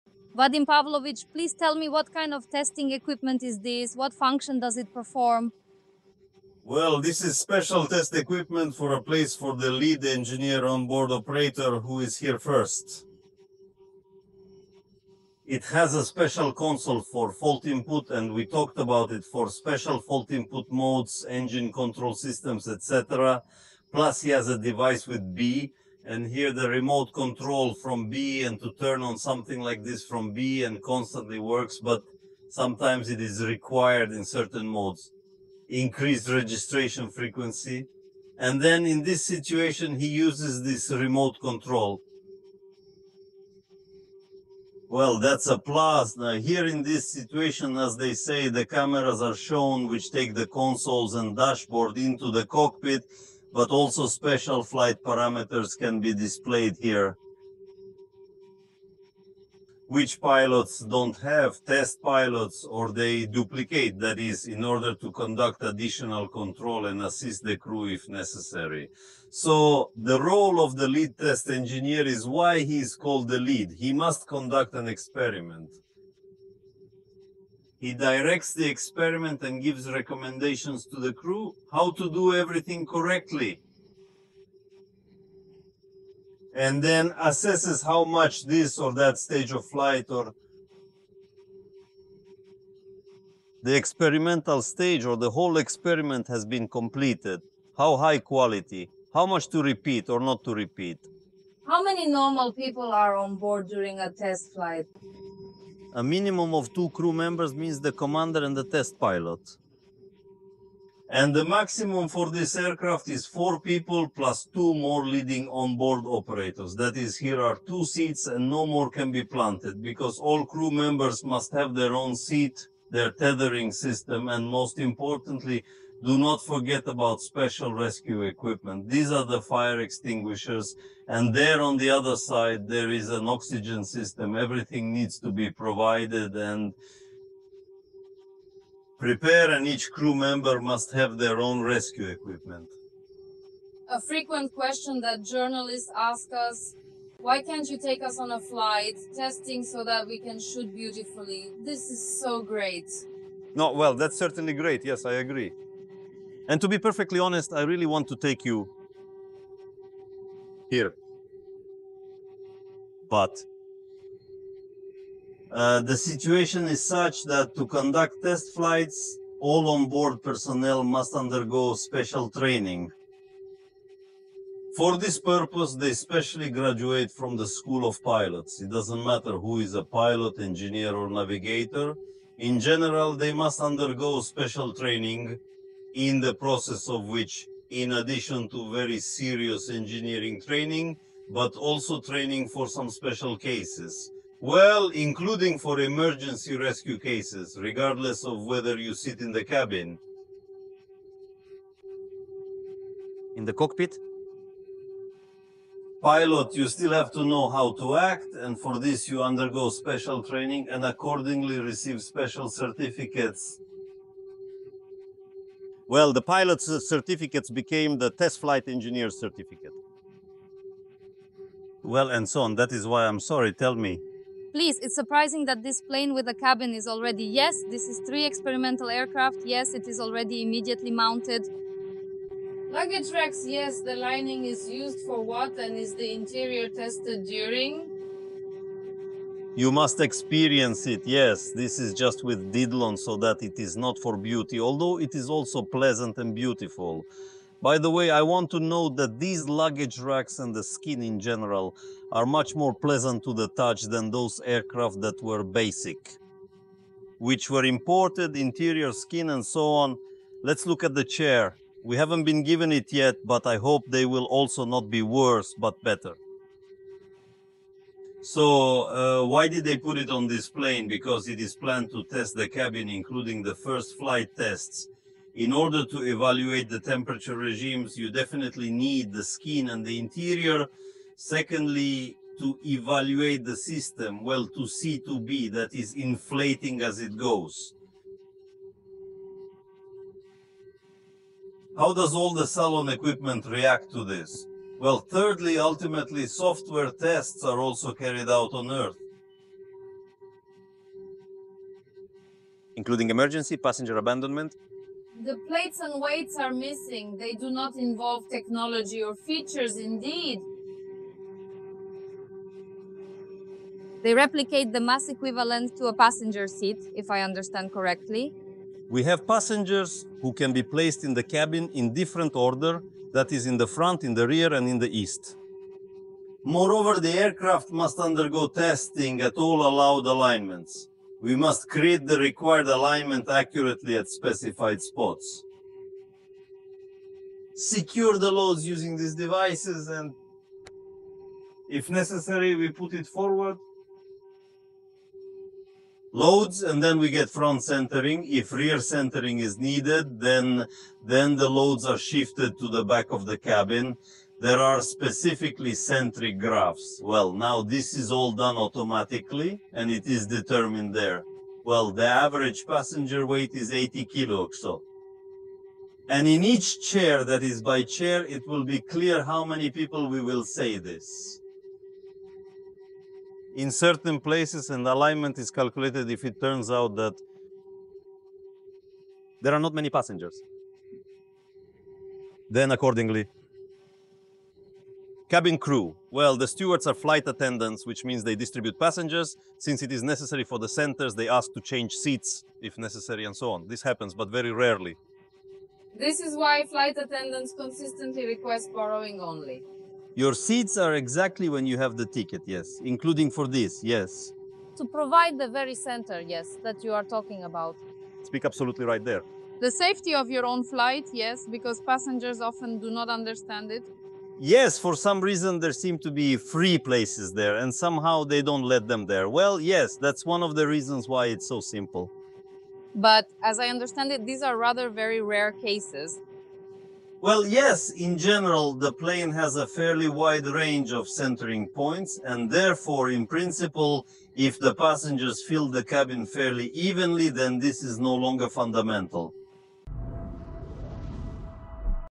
Pushing Superjet to BREAKING POINT: Russia's Extreme Flight Tests
What does it REALLY take to certify a passenger jet as safe? Join lead engineer Vadim Pavlovich for an exclusive look inside Russia's most extreme aircraft tests – where engineers purposely crash systems, fake passengers, and push jets beyond limits so YOU never have to experience disaster!

🔴 WATCH TO DISCOVER:
• The SECRET TEST CONSOLE that simulates mid-air emergencies
• Why journalists are BANNED from these dangerous flights
• How 80kg "fake passengers" help prevent real crashes
• The shocking reason cabin interiors are tested to destruction
• What happens when pilots purposely FAIL engines at takeoff

✈️ Most passengers will NEVER see what happens behind the scenes to keep flights safe!